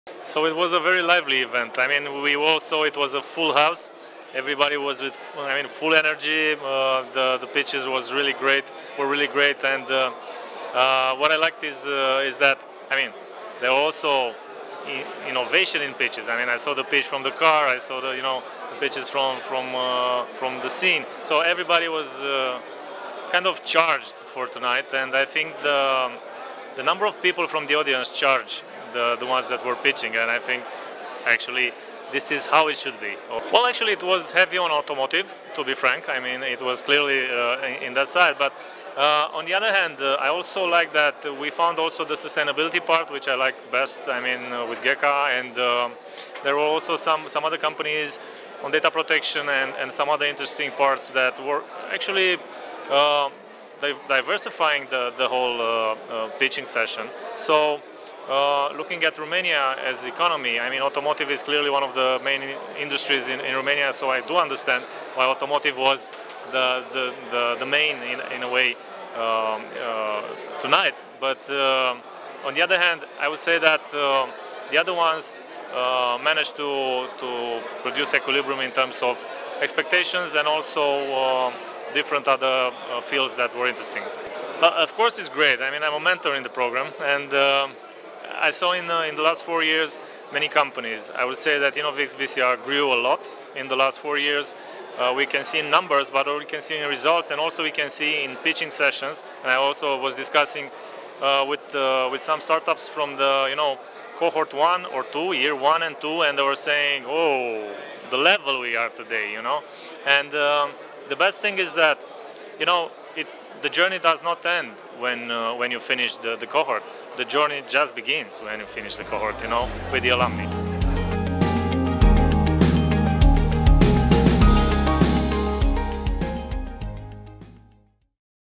Stefan Buciuc - Startups Alliance Demo Day Testimonial
We couldn't resist spending a little more time with our members of our program and asking them to give us some impressions at the final of this Demo Day. 😉

🤝We care about the experience of each of our participants and we are eager to find out what they have to say about their experience in our program. That is why we had Stefan Buciuc, CEO la BCR Social Finance IFN SA, answering to some of our curious questions! 👀

🔎 Watch this short video and discover what a star in the InnovX - BCR Startups Alliance Demo Day feels about us!🤩